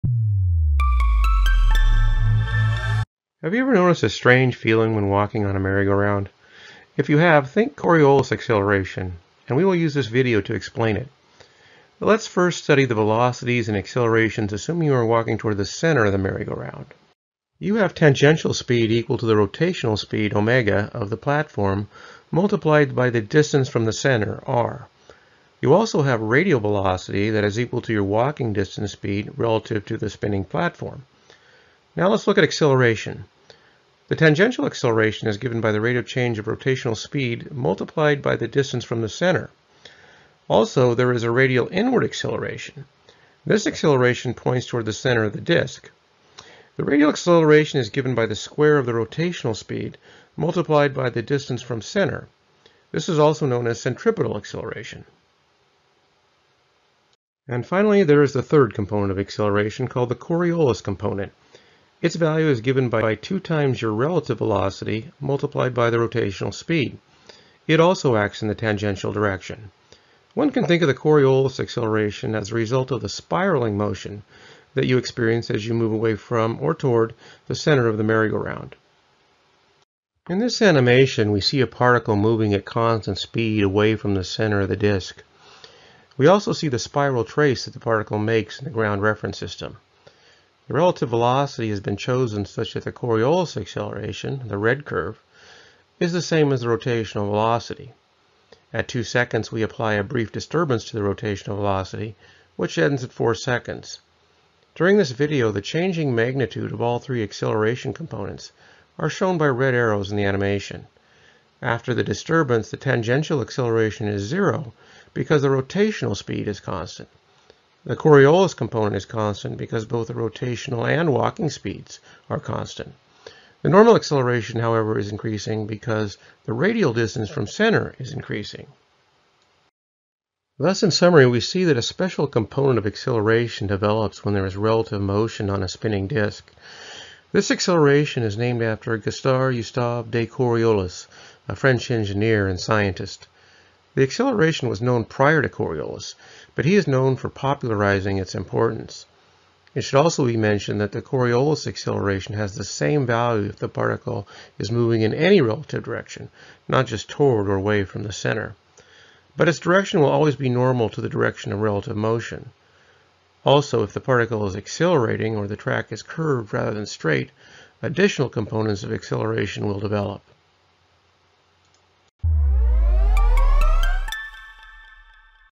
Coriolis Acceleration - A Walk on the Carousel
This video explains the Coriolis effect using the example of a ride on a carousel